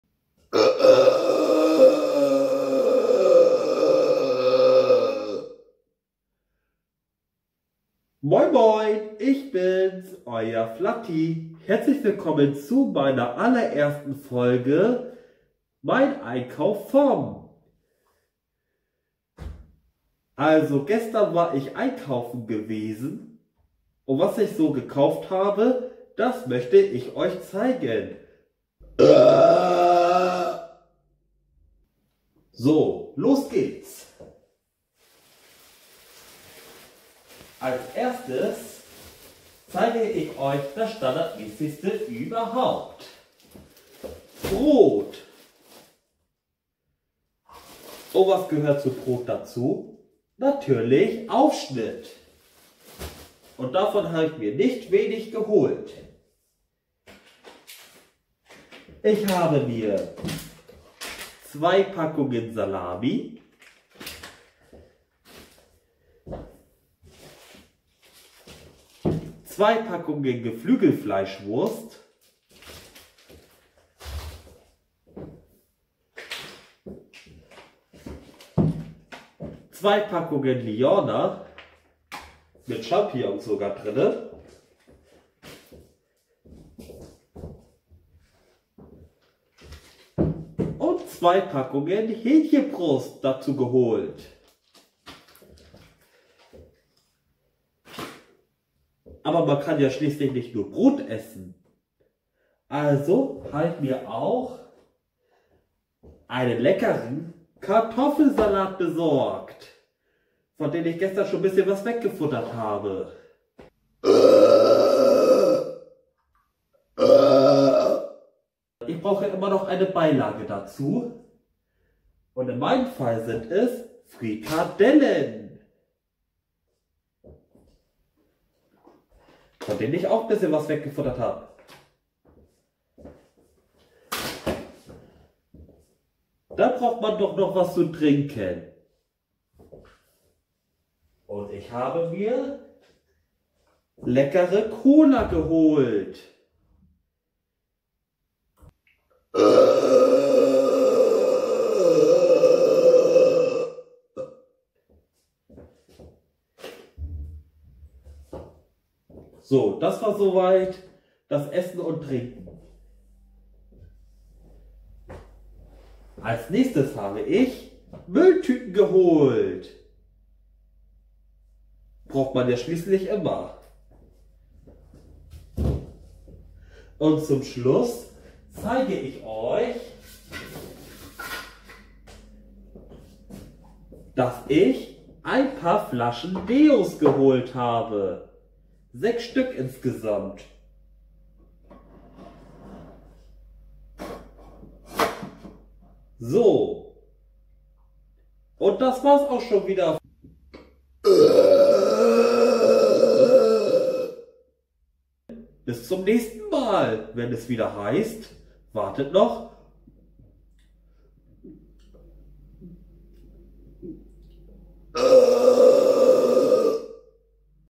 Mein Einkauf vom 15.06.2019
Gestern (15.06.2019) war ich einkaufen. Was ich mir geholt habe, zeige ich euch in diesem Video.

Kamera: Samsung Galaxy S9
Schnittprogramm: YouCut - Video Editor
Thumbnailprogramm: Thumbnail Maker